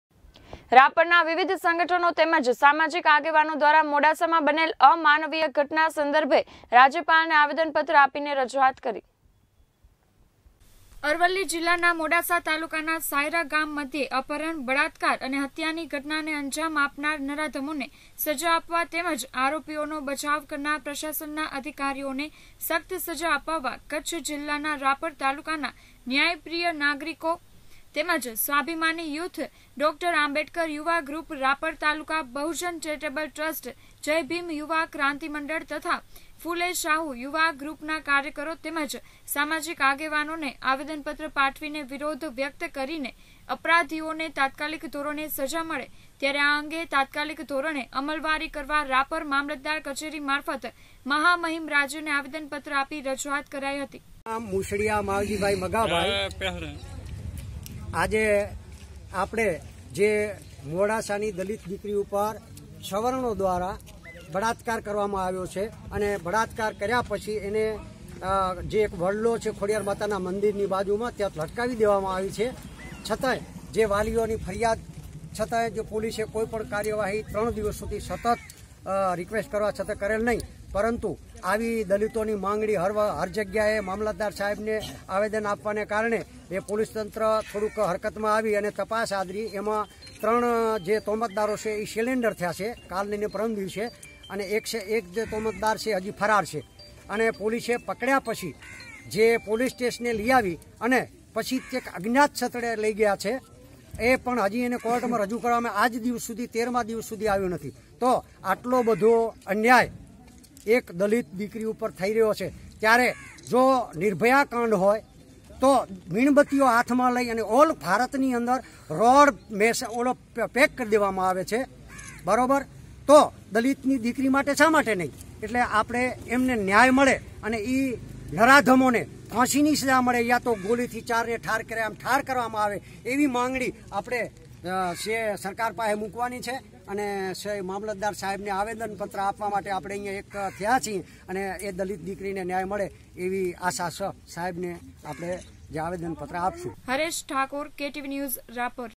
RAPAR: રાપરના વિવિધ સંગઠનો તેમજ સામાજિક આગેવાનો દ્વારા મોડાસામાં બનેલ અમાનવીય
RAPAR: રાપરના વિવિધ સંગઠનો તેમજ સામાજિક આગેવાનો દ્વારા મોડાસામાં બનેલ અમાનવીય ઘટના સંદર્ભે રાજ્યપાલને આવેદન પત્ર આપીને રજુઆત કરી